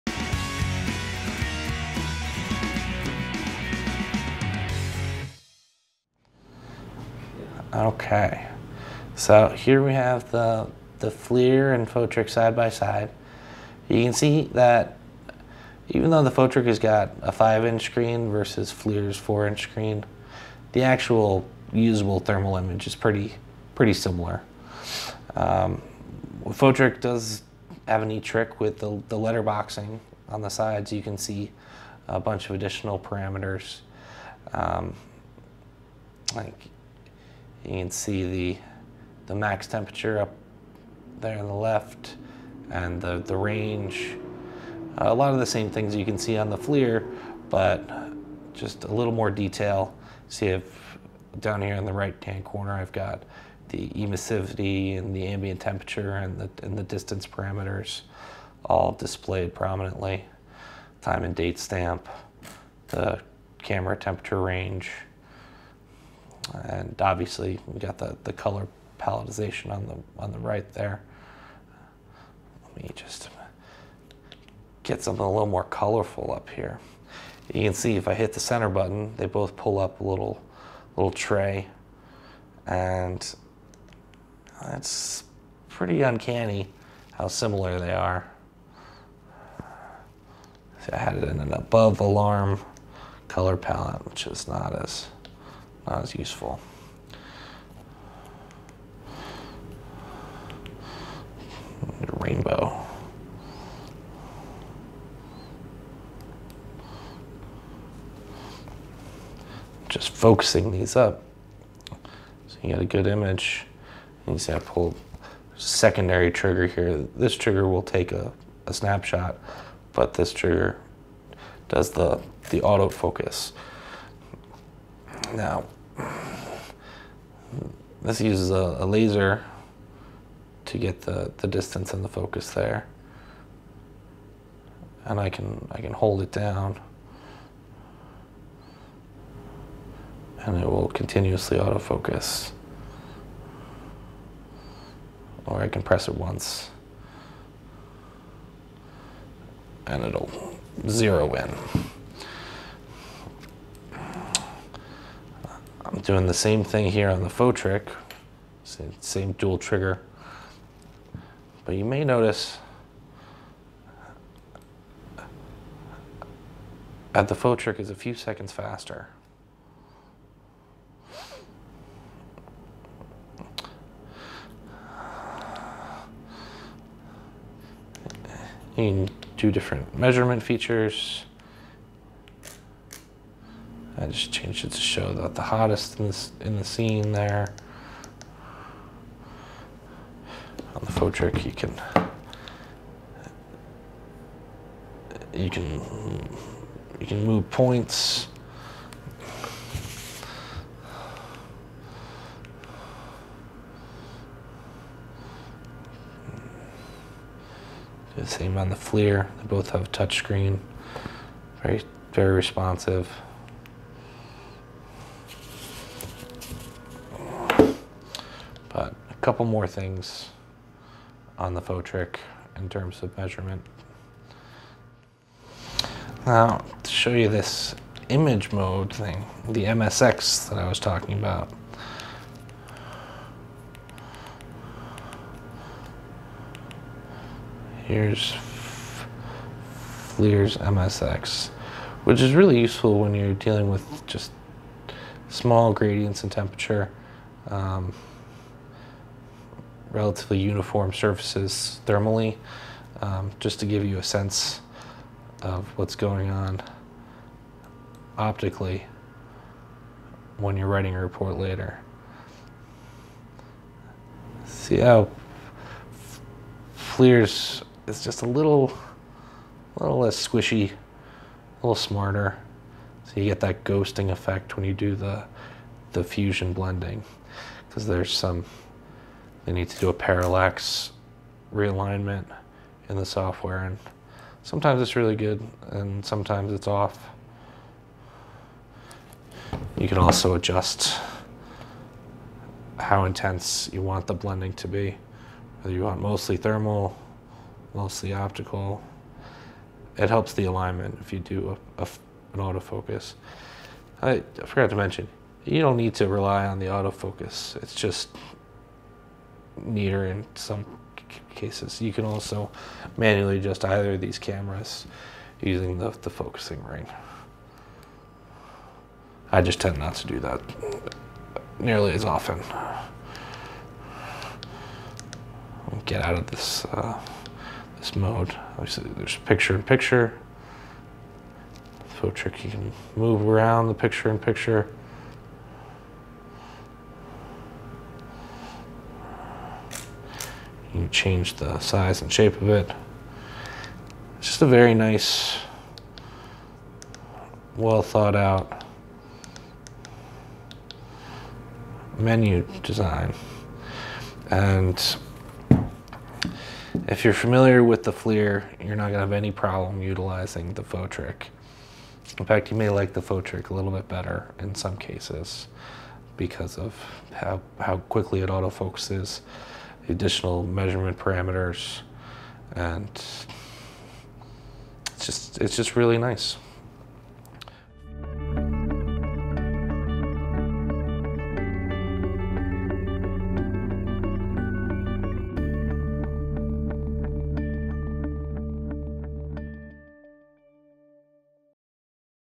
Fotric 348A vs FLIR Thermal Camera Comparison | Camera Demo
Screen Size: 00:13
Color Palettes & Menu Interface: 01:13
Auto Focus: 01:46
Measurement Modes: 02:57
Fotric Image Blending vs FLIR MSX: 03:45
Picture-in-Picture: 05:27

The Fotric 348A is a professional industrial thermal camera designed for maintenance, utilities, equipment surveys, and industrial inspections. In this video, Devin from TEquipment goes over all of the features and on-camera functions of the Fotric 348A and compares it to an equivalent FLIR camera, the E95 (now discontinued). The 348A has a very similar user interface and form factor as well as nearly all the same features as the E95.

The main advantage of the Fotric 348A is the laser-assisted AI auto focus. By using an AI algorithm, the 348A can auto focus several seconds faster than the E95. The E95 also has laser-assisted auto focus, but it is noticeably slower.

The biggest advantage for the E95 is FLIR MSX, a proprietary image blending technology. Fotric 348A does have image blending, but it is opacity-based, blending a visual and thermal image together using an overlay. FLIR MSX provides a much more detailed image.

Both cameras have a similar form factor and both have touchscreen displays. Fotric 348A has a 5-inch screen compared to a 4-inch screen on the E95. However, the usable thermal image is about the same size. Fotric applies a letterbox affect around the image which allows for more measurement parameters to be displayed.

Both cameras have a nearly identical user interface and menu structure. Pressing the center button on both cameras brings up the main menu tray where you can access measurement modes, image blending, color palettes, and more.

Overall, Fotric 348A is a strong and affordable option for professional thermography. It has nearly all the same features and functions as an equivalent FLIR camera, but it costs thousands of dollars less.